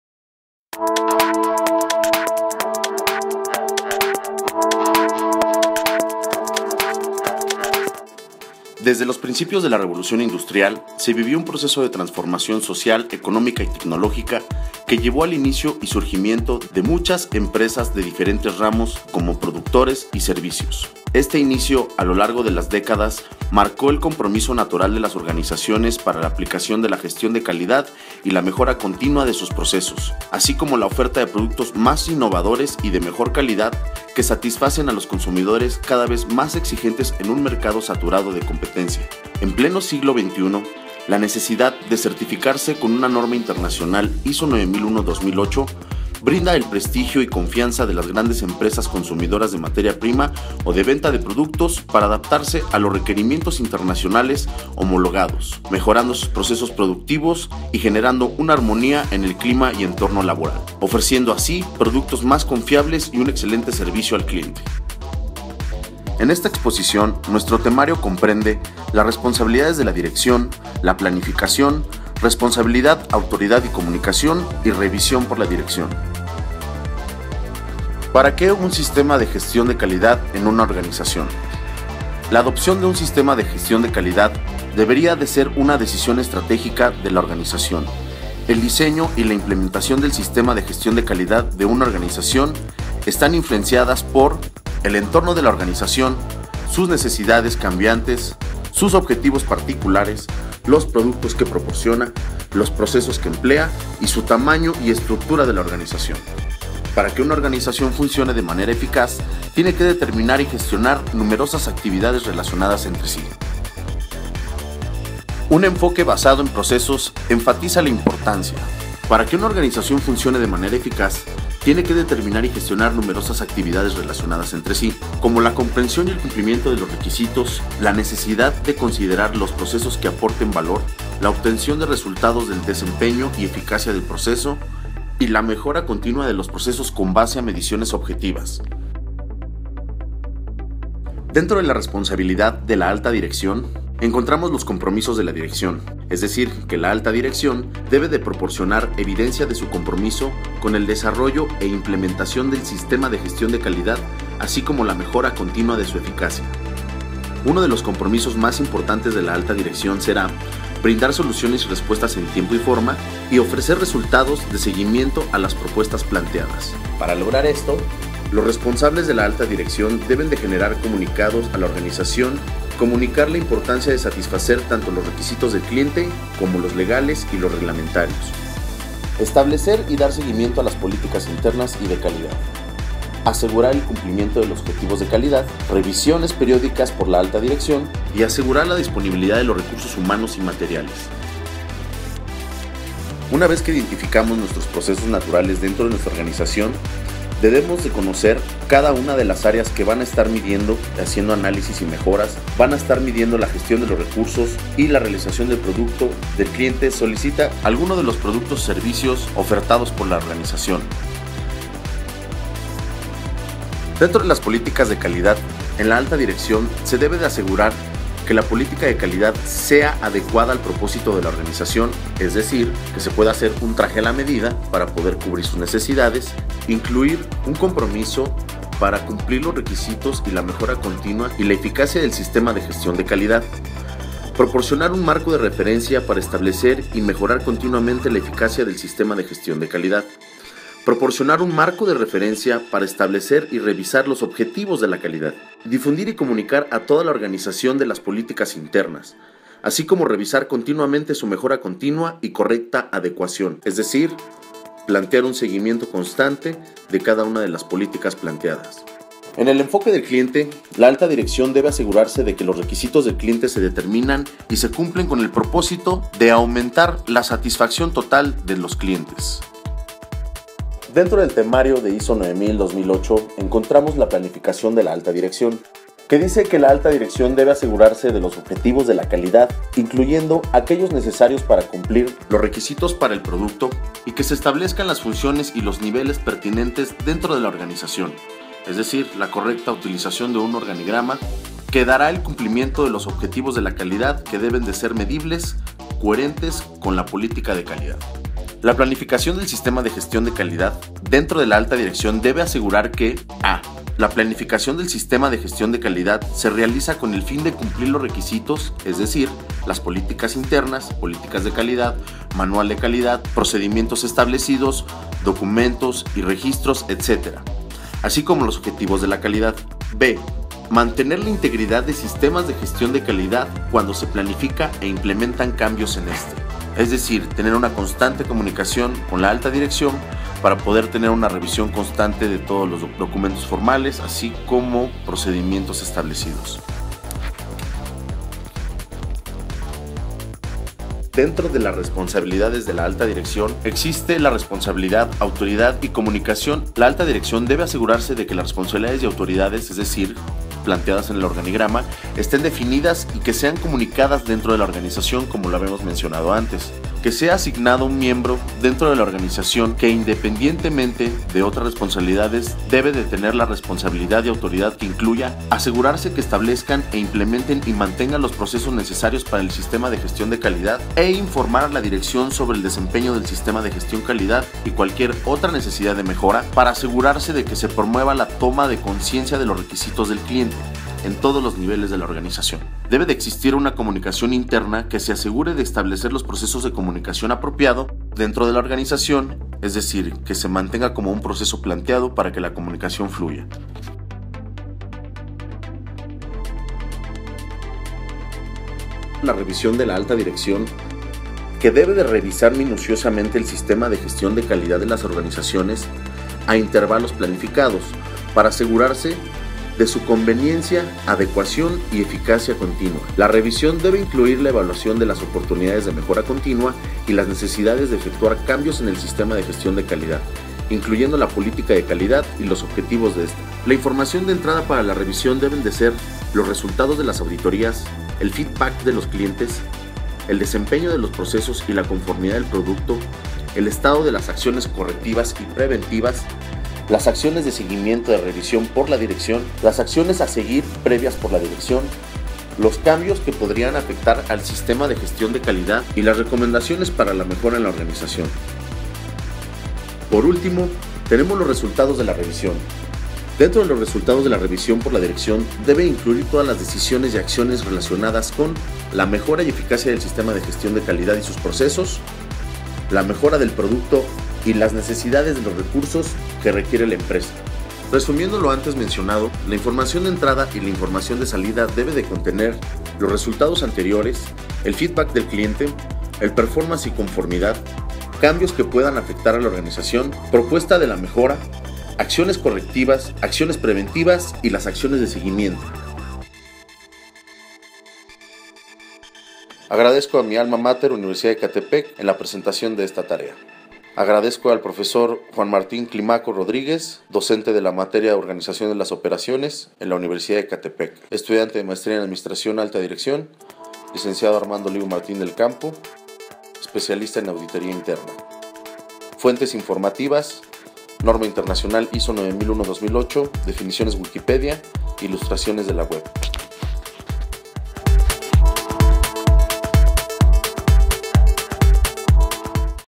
RESPONSABILIDADES DE LA ALTA DIRECCIÓN - ISO 9001-2008
Desde los principios de la Revolución Industrial, se vivió un proceso de transformación Social, Económica y Tecnológica, que llevo al inicio y surgimiento de muchas empresas de diferentes ramos (Productores, Servicios, etc). Este inicio a lo largo de las décadas marco el compromiso natural de las organizaciones por la aplicación de Gestión de Calidad y Mejora continua en sus procesos, y la oferta de productos más innovadores y de mejor calidad, que satisfacen a los consumidores cada vez más exigentes en un mercado saturado de competencia en la actualidad.

En pleno siglo XXI, la necesidad de certificarse en una NORMA INTERNACIONAL ISO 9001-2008, brinda el PRESTIGIO y CONFIANZA de las grandes empresas consumidoras de materia prima o de venta de productos para adaptarse a los requerimientos internacionales y homologados, ofreciendo productos más confiables y un excelente servicio al cliente.

Mejorando sus procesos productivos y generando armonía
en clima y entorno laboral.

L.C.C. Armando Olivo Martín del Campo
Auditor Interno SGC ISO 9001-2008
Tecnología e Inteligencia Activa Euromex, SAPI de C.V.

NOTA: La información proporcionada fue tomada de la NORMA INTERNACIONAL de Sistemas de Gestión de Calidad ISO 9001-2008, algunos contenidos fueron complementados e ilustrados para hacer más claro su entendimiento y reforzamiento educativo.

ESTUDIANTE MAESTRÍA EN ADMÓN. ALTA DIRECCIÓN:
Alma Mater: UNIVERSIDAD DE ECATEPEC
(Edición, Redacción, Locución, Diseño Presentación y Especialista en Auditoría Interna)